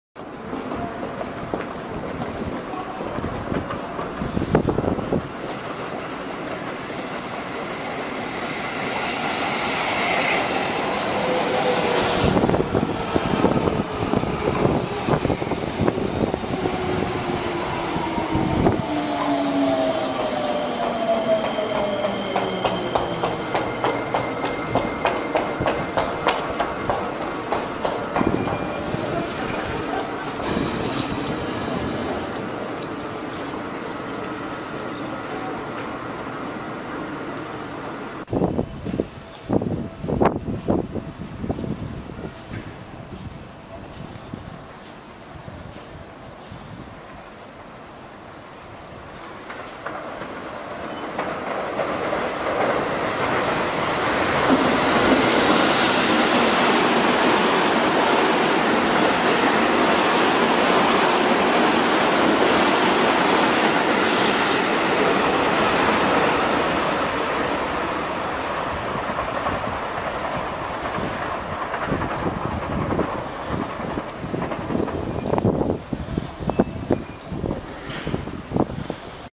環八の隣にある駅
西武新宿線井荻駅です。環八通りこと、東京都道311号環状8号線の東隣に駅があります。

跨線橋の上から撮影した西武20000系電車は、列車番号　5128　各駅停車　田無11:41発　井荻11:55着　11:56発　西武新宿12:15着です。目の前には環八通りが西武新宿線を跨ぐ井荻陸橋があります。この地下には井荻トンネルがあります。

この井荻陸橋と井荻トンネルができる前は環八通りと西武新宿線は踏切（井荻踏切、井荻第1号踏切）で平面交差しており、年から年中渋滞していました。

0:38頃にやってくる西武30000系電車は、列車番号　2656　急行　本川越11:17発　西武新宿12:17着です。井荻駅は通過します。

2013年12月22日撮影。